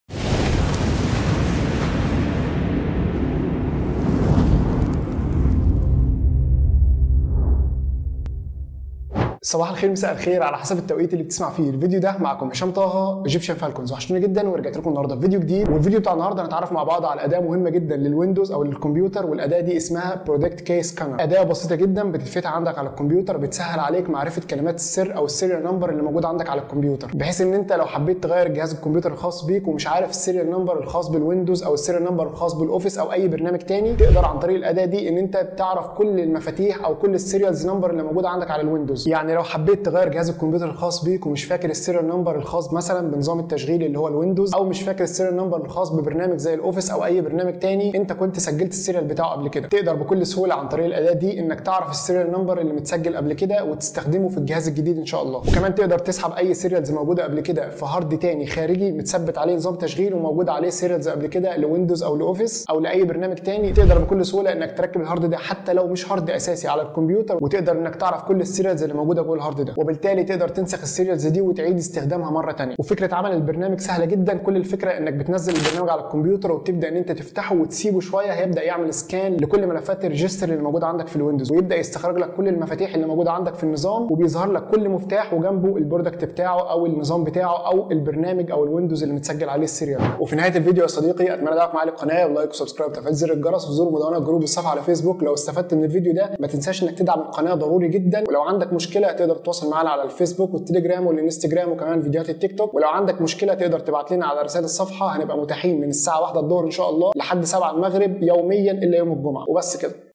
شرح اداة فحص السيريالات Product Key Scanner | How to Find Product Key
تقدم لكم قناة egyptian falcons الصقور المصرية شروحات اداة فحص السيريالات على الويندوز productkeyscanner

00:00 المقدمة
00:33 مميزات الاداه
01:13 شرح الاداه بالتفصيل على الكمبيوتر
01:30 النهاية